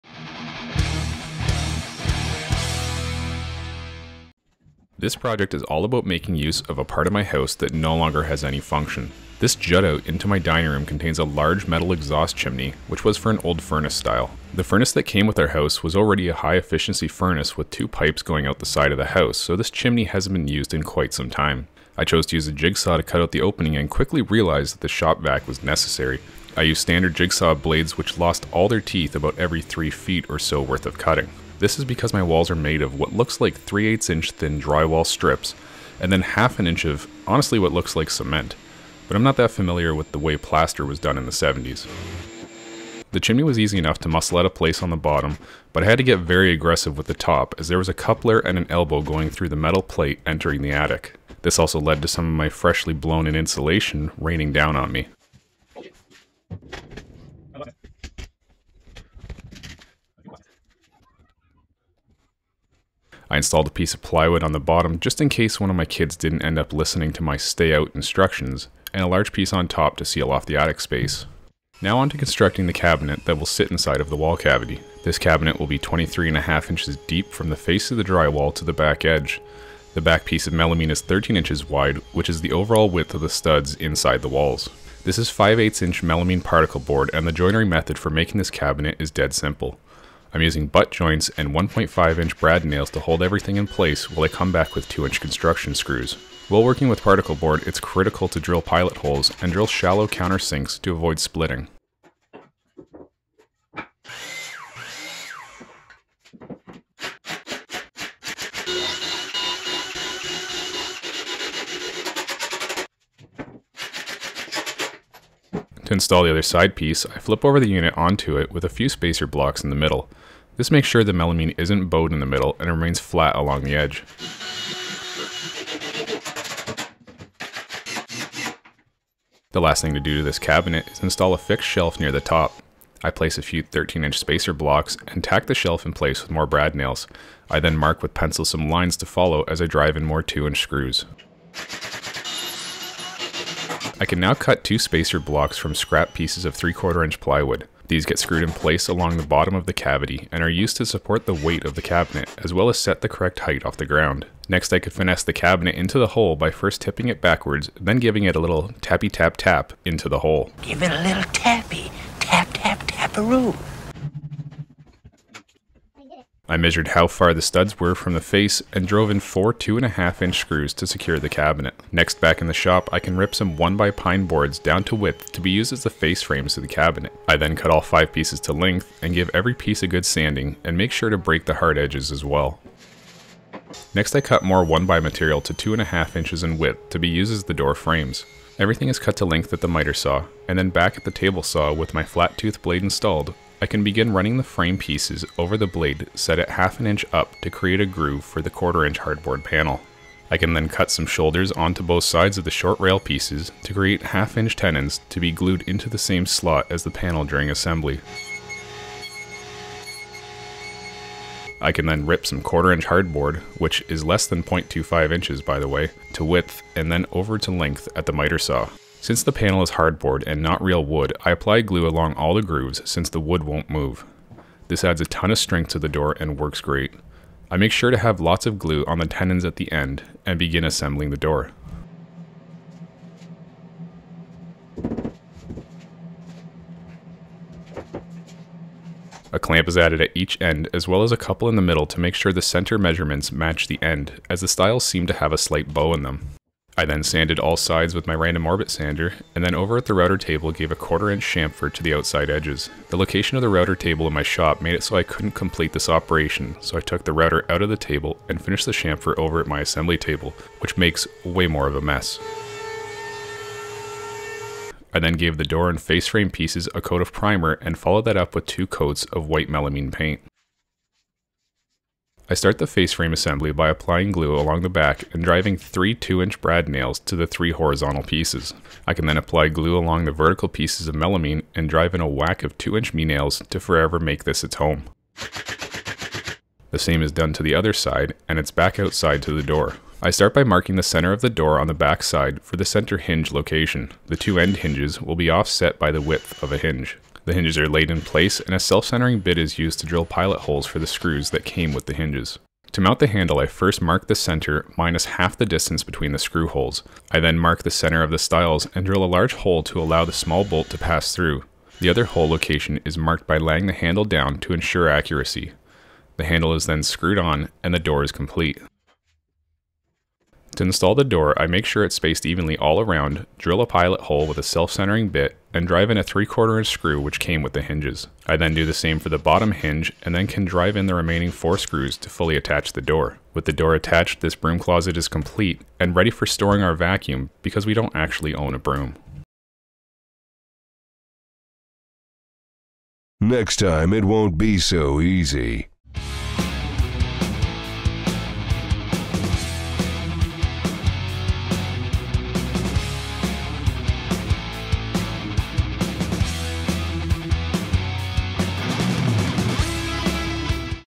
What's Inside the Wall? DIY Broom Closet Built-in
I made use of a section of my house that was previously wasted space. This broom closet is perfect for storing our cleaning supplies and is free real-estate.